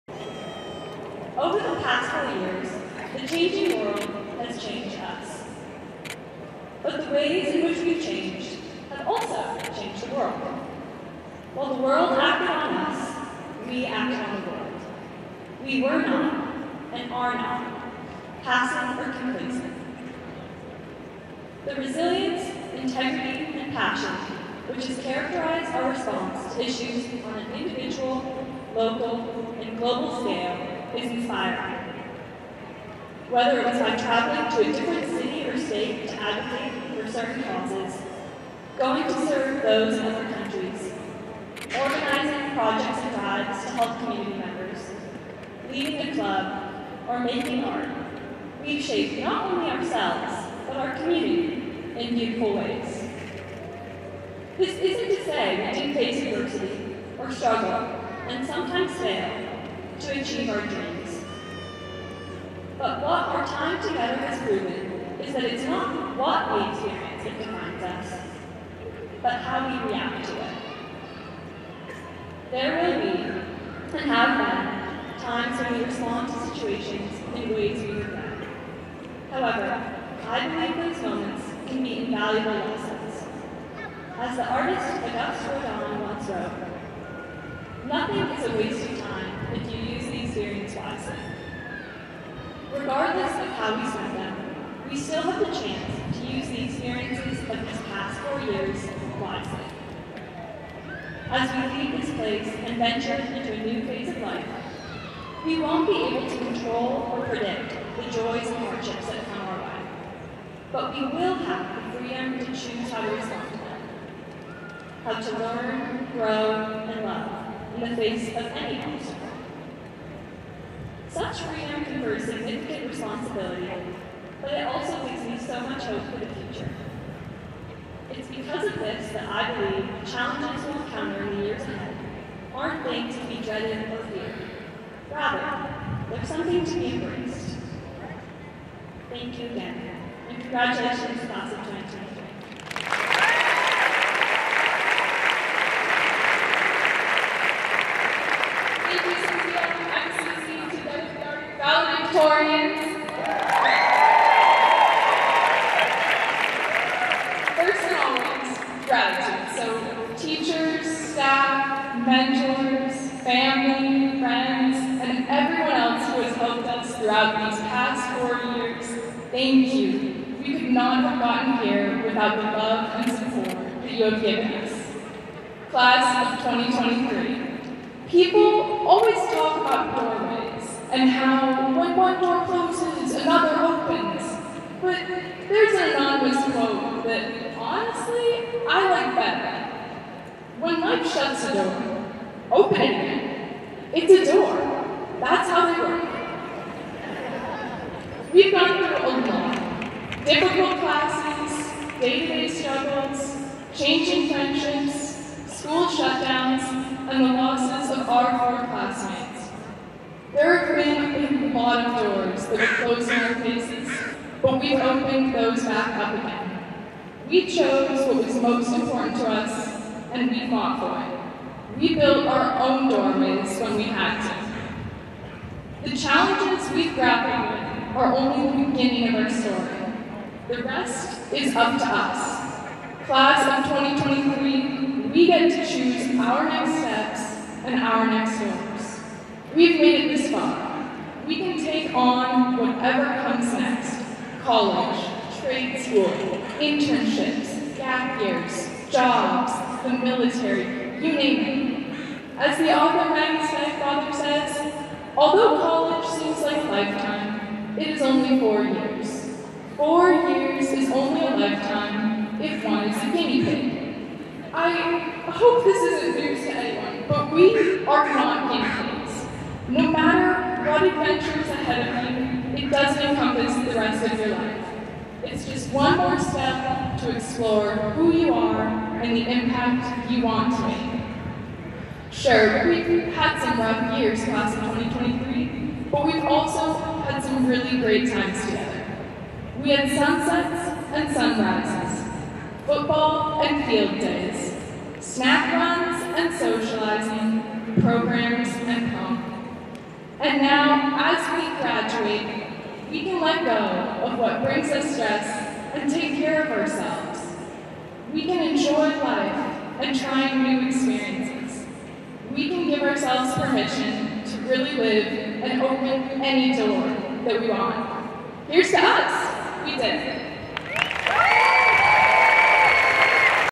WSHS 2023 graduation: Valedictorians
West Seattle Blog video of Sophia Butler and Cece Pyfer